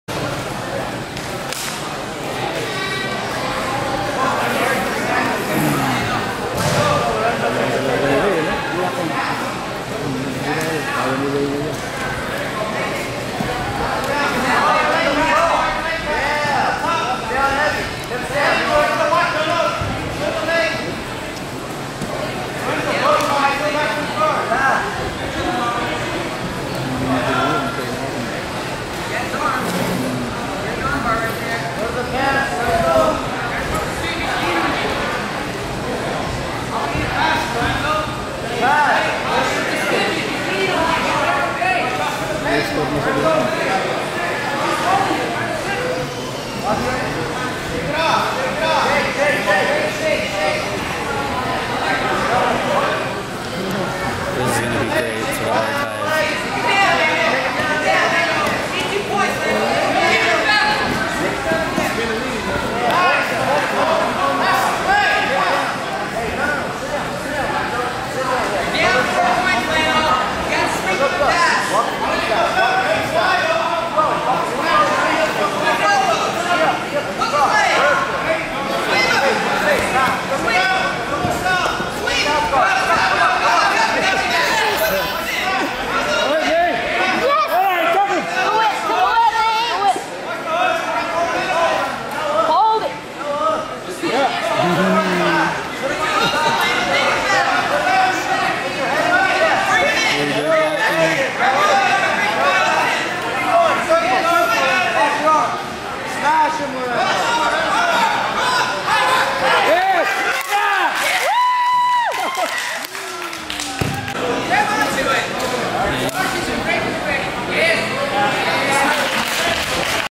BJM Classic BJJ Tournament (2008): White Belt Finals
Big John McCarthy BJJ Tournament (2008)
White Belt Finals

First time winning Gold at a Tournament.